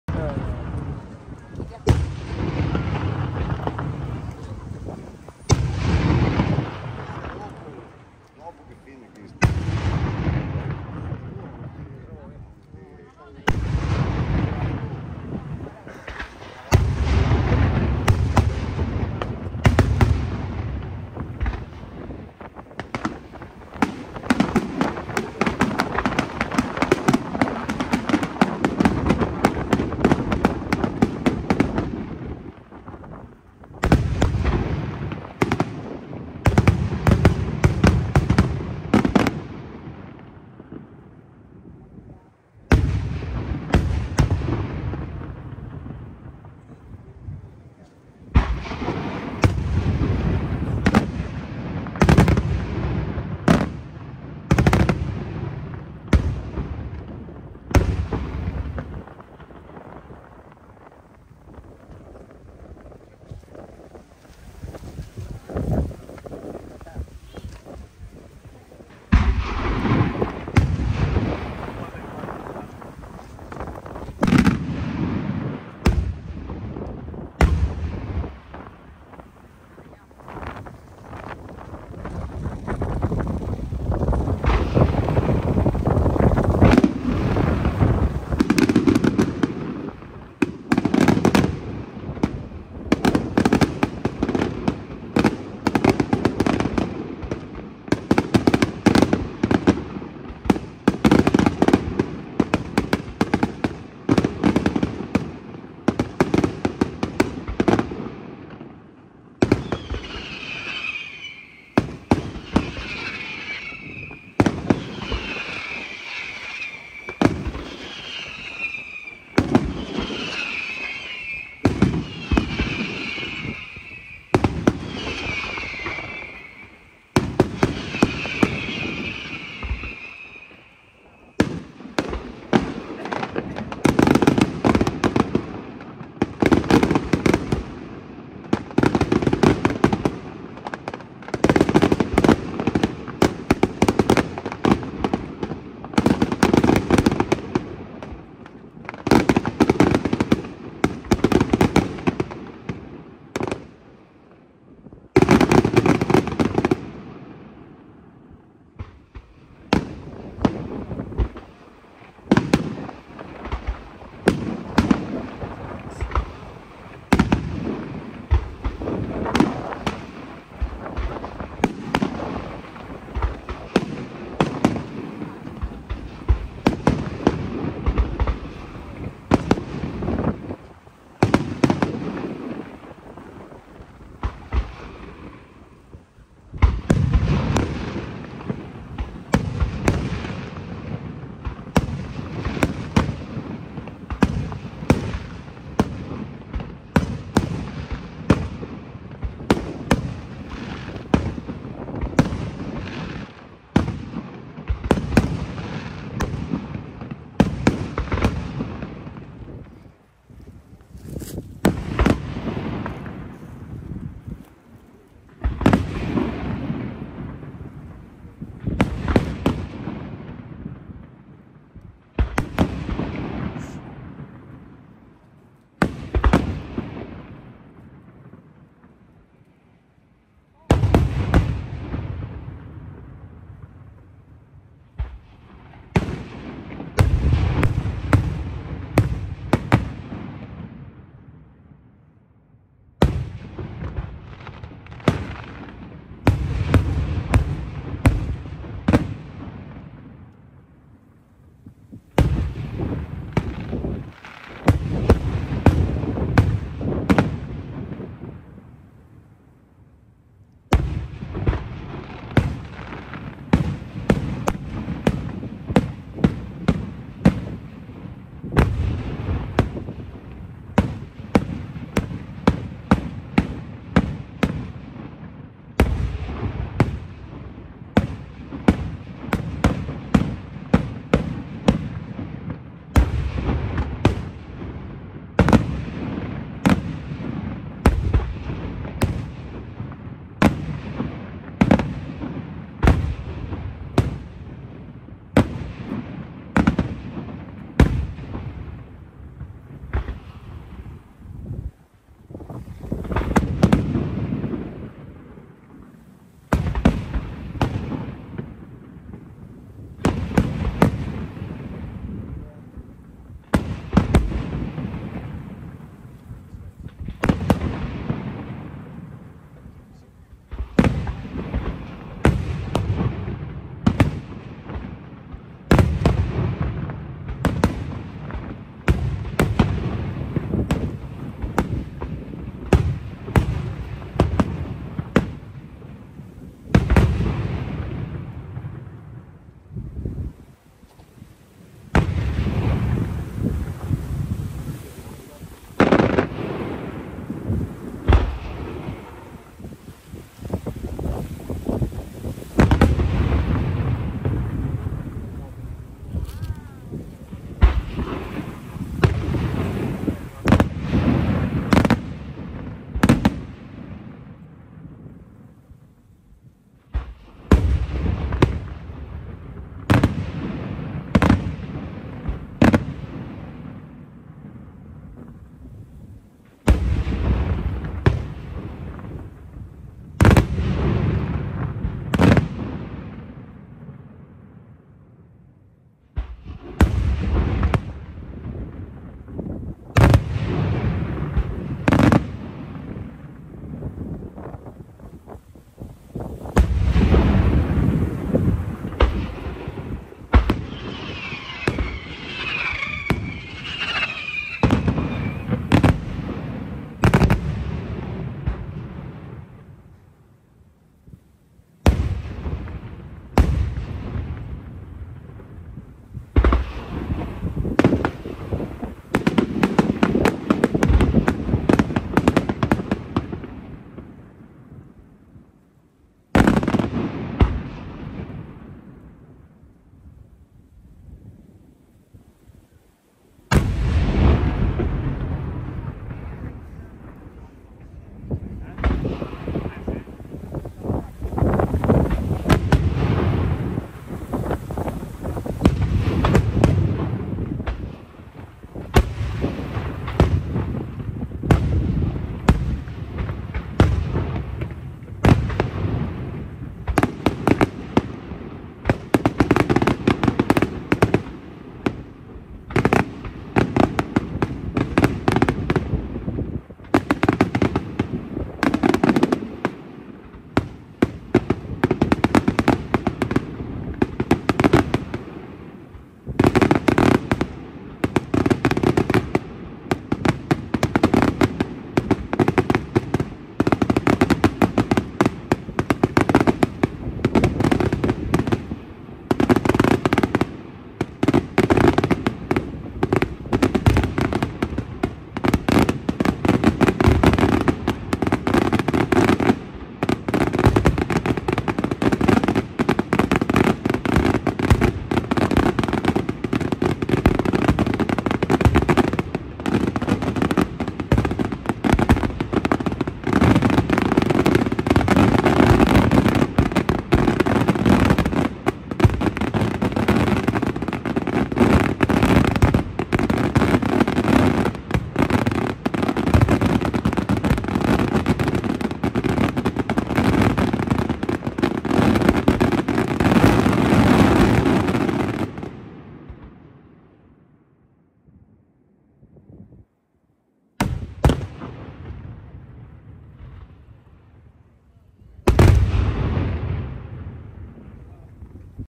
Benestare (Rc) Maria Santissima del Rosario 2024 PIROTECNICA F.lli ROMANO
Benestare (Rc) 06 Ottobre 2024
Festeggiamenti in onore di Maria Santissima del Rosario 2024
Tradizionale Festival Pirotecnico
A cura della ditta
PIROTECNICA F.lli ROMANO da Angri (Sa)
Riprese video: Fabrizio Parisi